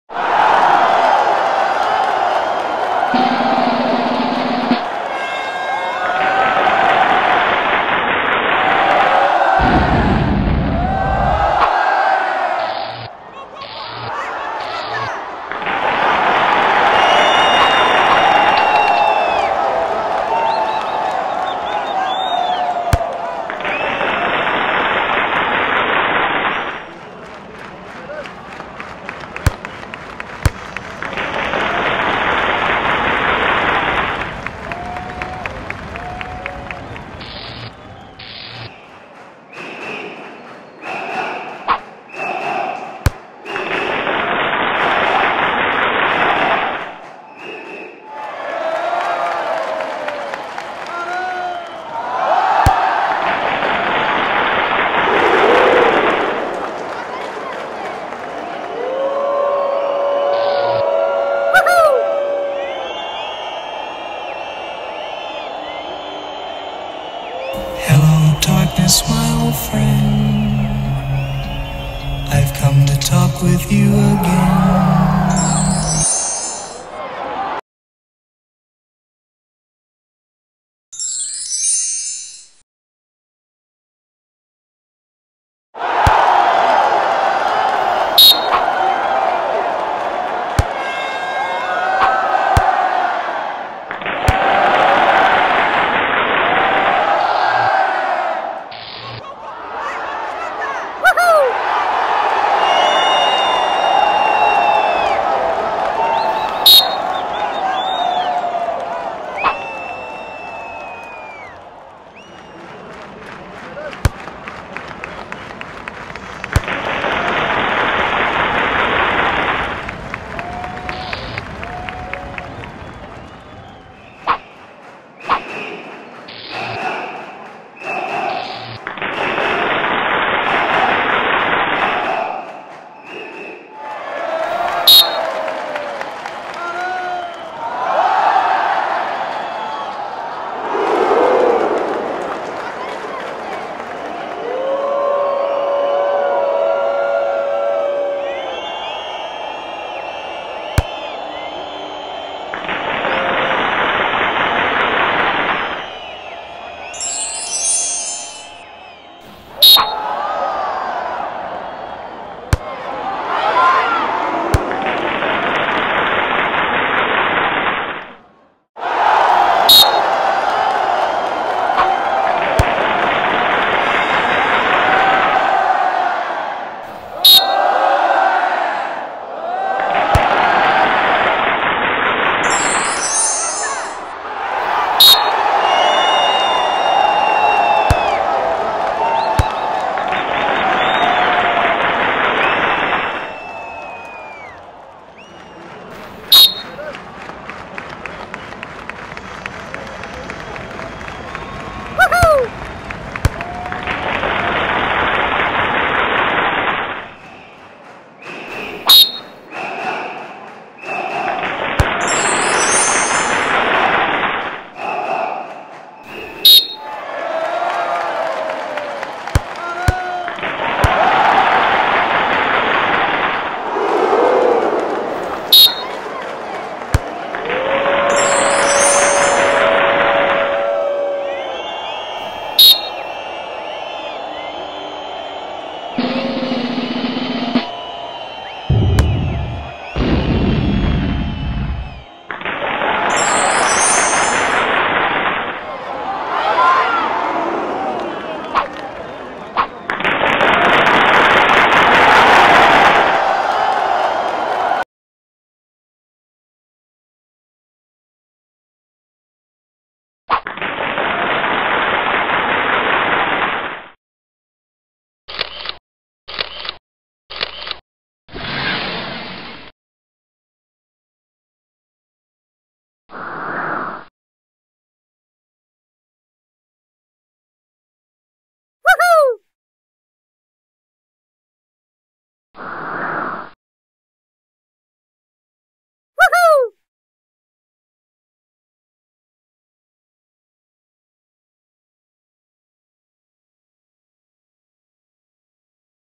Guinea Pig Soccer Showdown
Ponyo and Totoro battle it out in this silly head-to-head soccer match :)